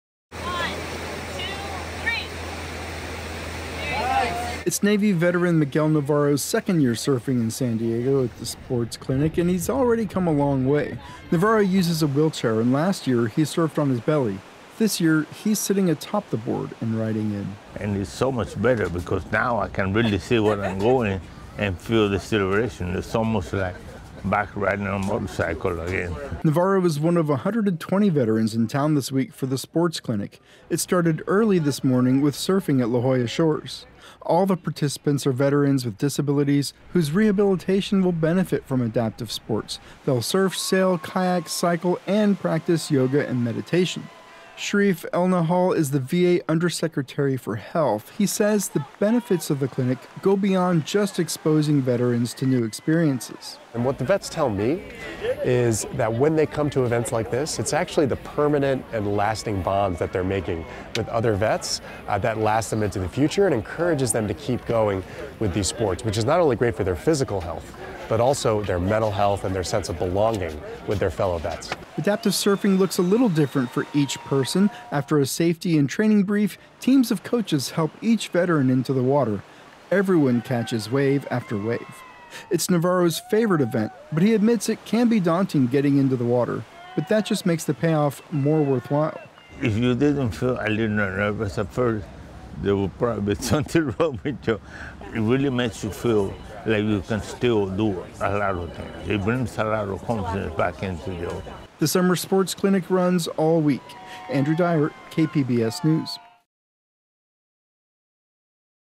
Veterans drop into week-long adaptive sports clinic surfing in La Jolla
Veterans from all over the country are in San Diego this week for a series of adaptive sports events. It’s all part of the VA’s Summer Sports Clinic. KPBS military and veterans reporter Andrew Dyer caught up with them at a morning surf session.